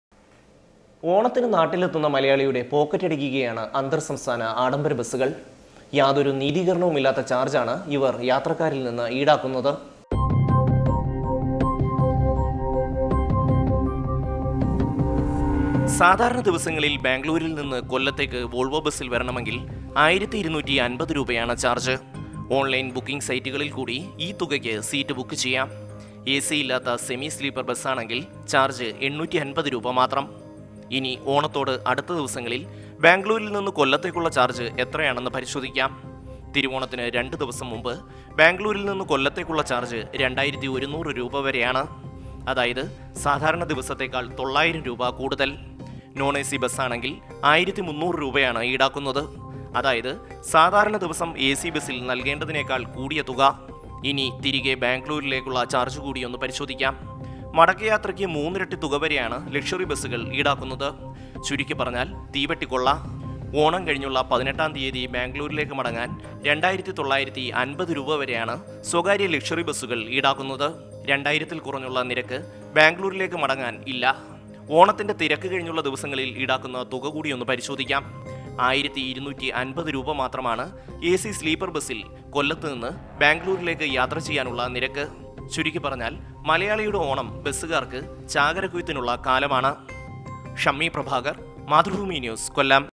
ഓണക്കാലത്ത് യാത്രക്കാരെ കൊള്ളയടിക്കാന്‍ വോള്‍വോ ബസ് സര്‍വ്വീസുകാര്‍
കൊല്ലം: ഓണക്കാലമായതോടെ അന്തര്‍ സംസ്ഥാന സര്‍വ്വീസ് നടത്തുന്ന ആഡംബര ബസ്സുകള്‍ യാത്രക്കാരെ കൊളളയടിക്കുന്നു. ആയിരം രൂപ വരെ അധികം ഈടാക്കിയാണ് ഓണക്കാലത്ത്  ബസ്സുകള്‍ യാത്രക്കാരെ കൊണ്ട് വരുന്നത്. സാധാരണ ദിവസങ്ങളില്‍  ബംഗളൂരില്‍ നിന്ന് കൊല്ലത്തേക്ക് വോള്‍വോ ചാര്‍ജ്ജ് 1250 രൂപയാണ്. ഇത് ഓണ ദിവസങ്ങളില്‍ 2100 രൂപ വരെയാണ്. ഓണം കഴിഞ്ഞ് മടക്കയാത്രക്ക് മൂന്നിരട്ടി നല്‍കേണ്ടി വരും. 2950 രൂപവരെയാണ് തിരികെ പോകാനുള്ള ചാര്‍ജ്. ഓണം കഴിഞ്ഞുളള ദിവസങ്ങളില്‍ രണ്ടായിരത്തില്‍ കുറഞ്ഞുളള നിരക്കുമില്ല.